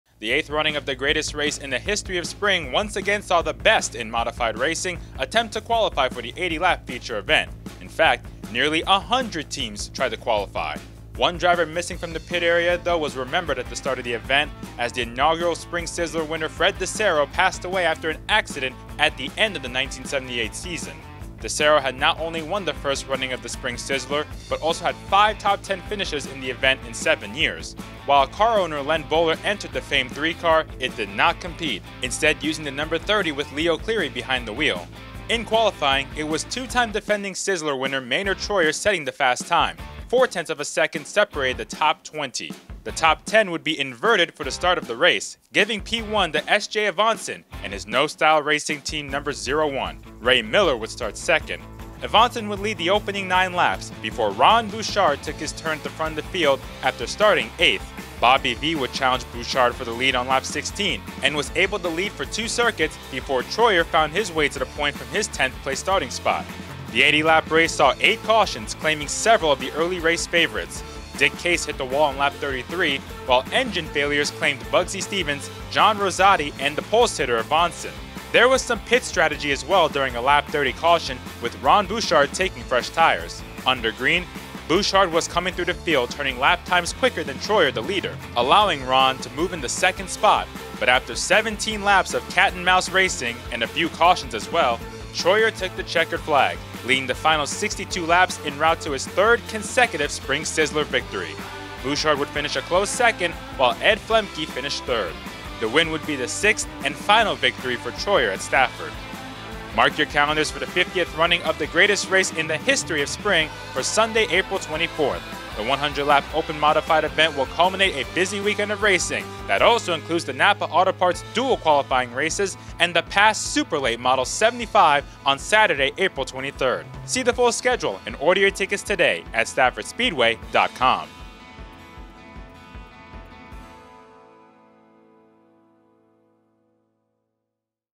Sizzler Shorts - 1979 Spring Sizzler®
🧨SIZZLER SHORTS

1979 - The 8th Spring Sizzler was a historic one as Maynard Troyer held off Ron Bouchard to become the first driver to win 3 Sizzlers in a row.

🗓️ 50th NAPA Auto Parts Spring Sizzler, April 23rd & 24th